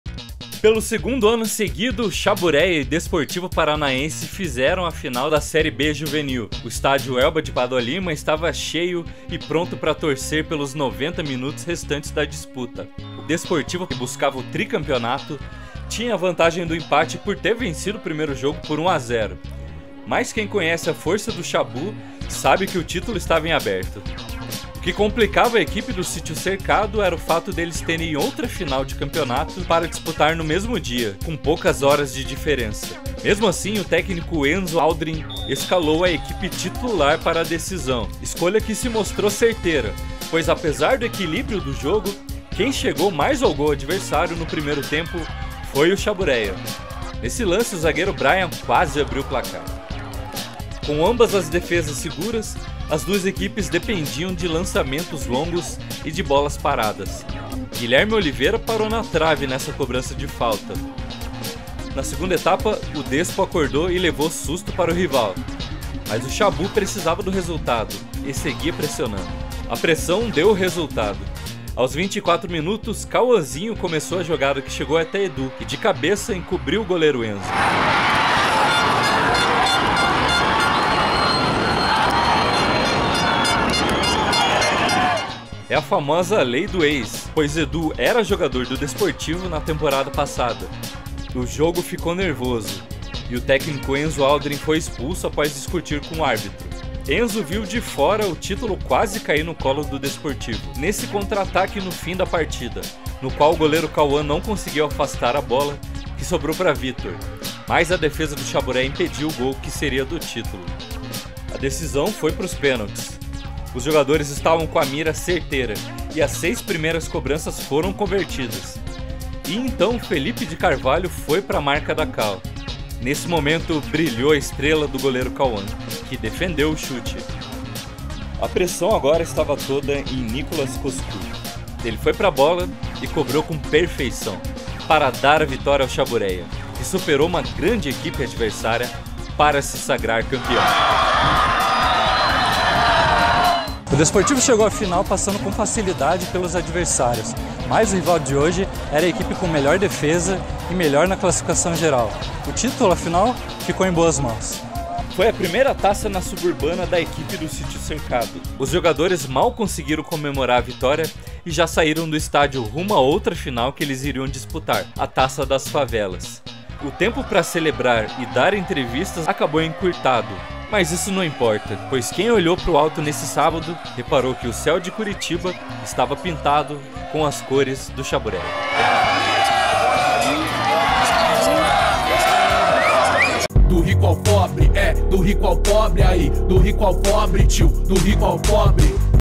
[VT] - SHABUREYA 1 X 0 DESPORTIVO [FINAL - VOLTA | SUBURBANA SÉRIE B | JUV]
Confira a reportagem DRAP da partida entre Shabureya e Palmeirinha, disputada no último sábado (18) no Estádio Elba de Pádua Lima. O duelo foi válido pela partida de volta da fase final da Série B da Suburbana 2023, categoria juvenil. Reportagem/Imagens/edição Daniel Palma |